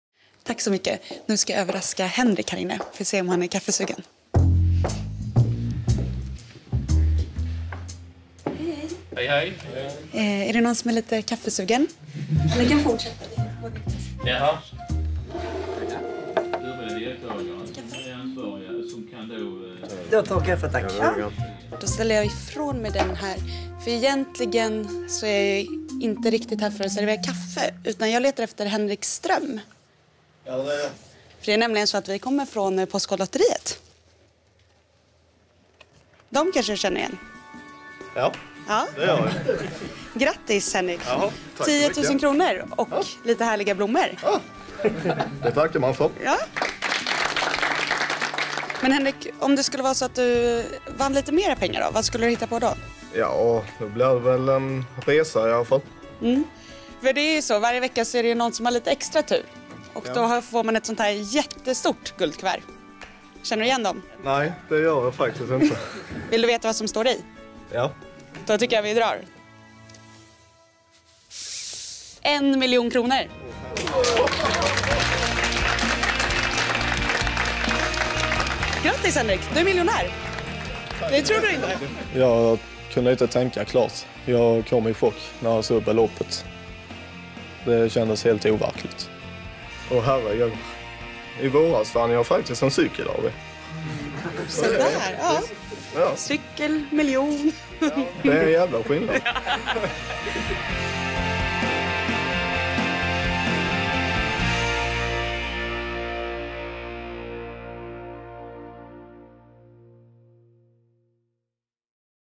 Två miljonärer i Knislinge
Veckans miljonvinnare heter Maj-Lis och Henrik.